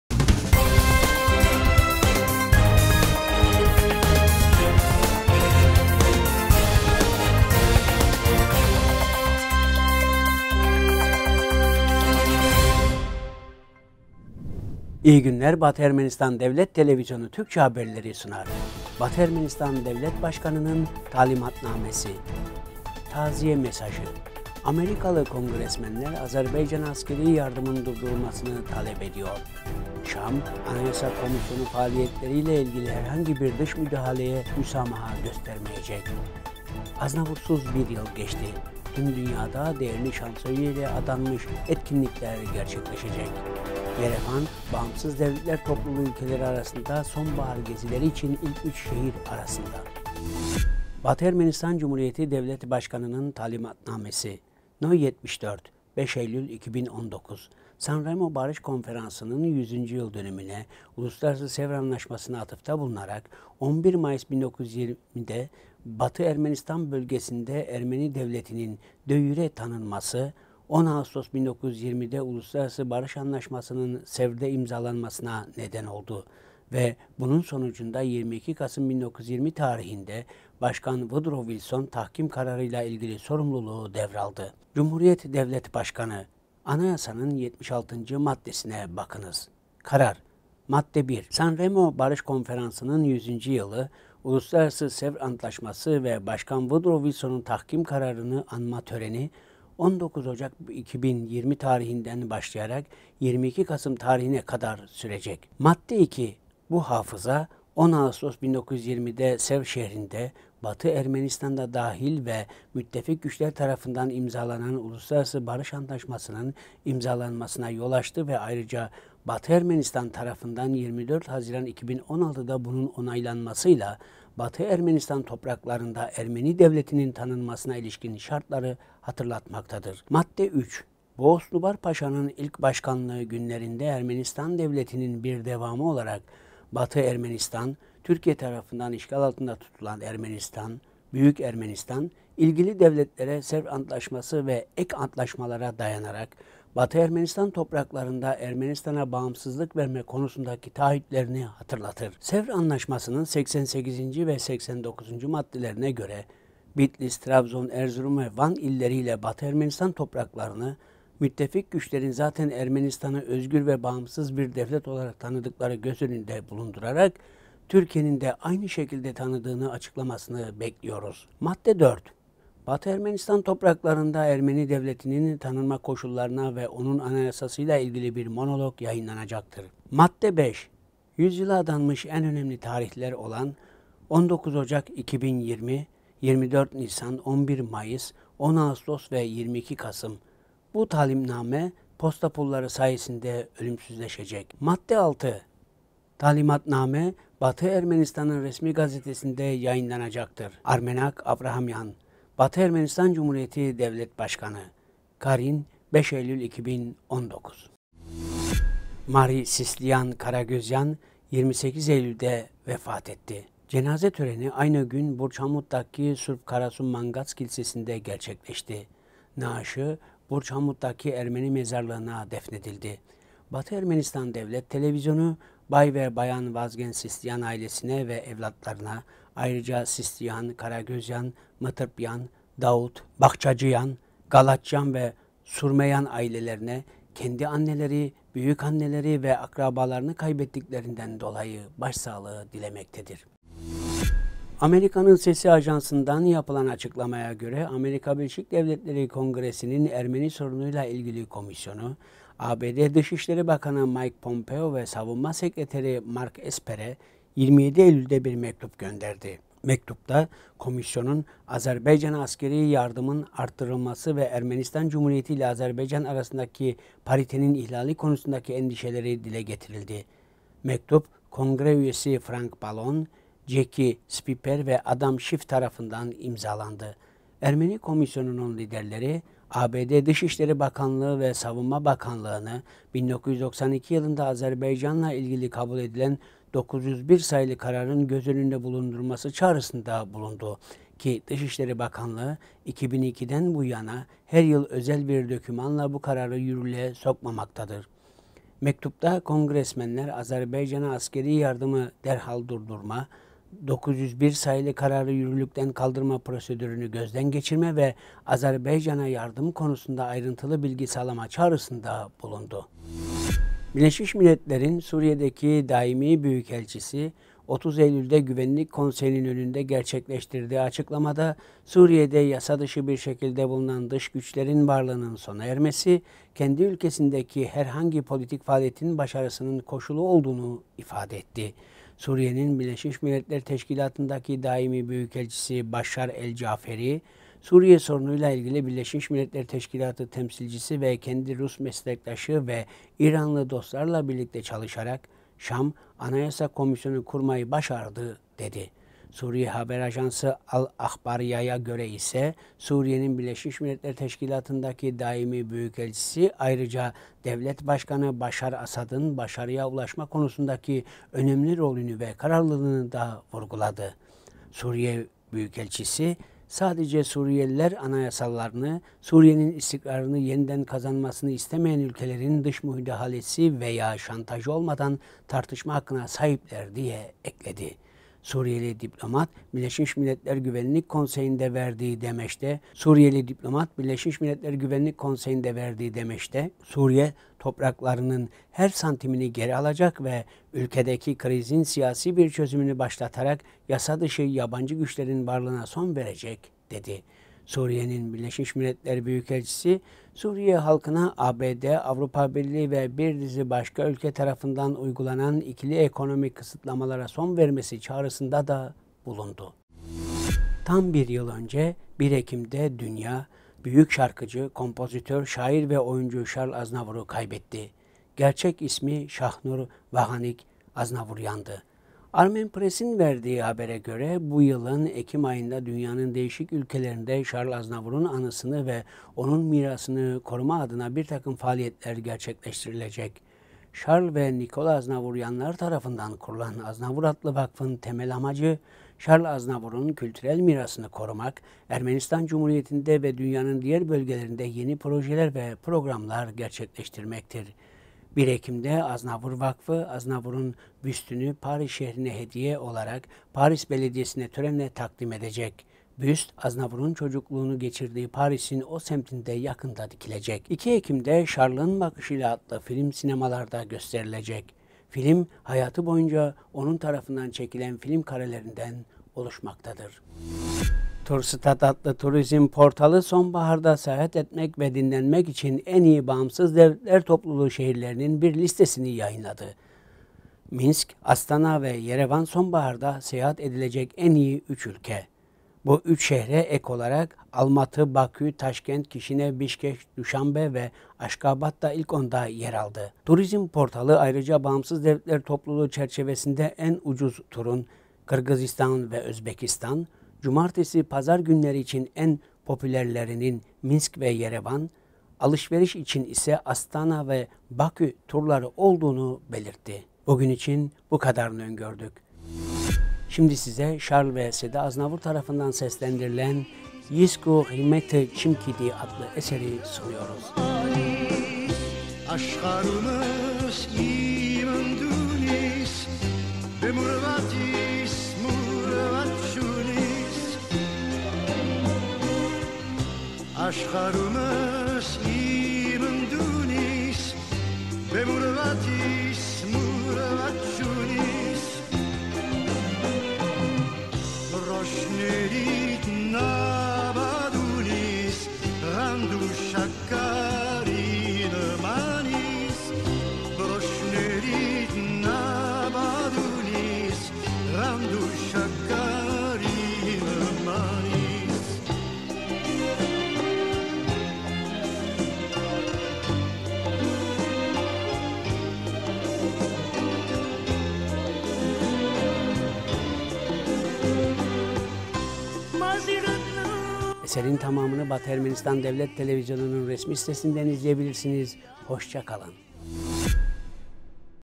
Batı Ermenistan Devlet Başkanının talimatnamesi .Haberler 2019-10-01
1- Batı Ermenistan Devlet Başkanının talimatnamesi
2- Taziye mesajı
3- Amerikalı kongresmenler Azerbaycan’a askeri yardımın durdurulmasını talep ediyor
4- Şam, Anayasa Komisyonu faaliyetleriyle ilgili herhangi bir dış müdahaleye müsamaha göstermeyecek
5- Aznavur’suz bir yıl geçti. Tüm dünyada değerli şansonyeye adanmış etkinlikler gerçekleşecek
6- Yerevan BDT ülkeleri arasında sonbahar gezileri için ilk üç şehir arasında